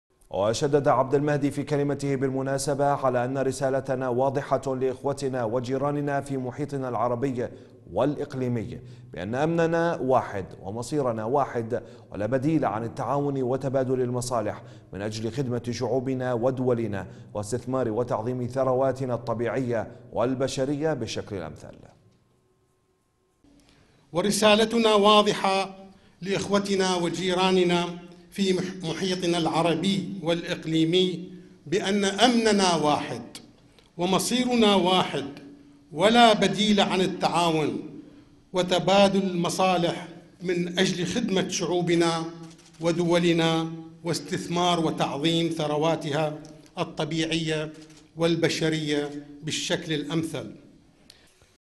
عبد المهدي بكلمة النصر رسالتنا واضحة لاخوتنا وجيراننا في محيطنا العربي والاقليمي بأن امننا واحد
عبد المهدي بكلمة النصر
رسالتنا واضحة لاخوتنا وجيراننا في محيطنا العربي والاقليمي بأن امننا واحد

وشدد عبد المهدي في كلمته بالمناسبة على ان رسالتنا واضحة لاخوتنا وجيراننا في محيطنا العربي والاقليمي بأن امننا واحد ومصيرنا واحد ولابديل عن التعاون وتبادل المصالح من اجل خدمة شعوبنا ودولنا واستثمار وتعظيم ثرواتها الطبيعية والبشرية بالشكل الامثل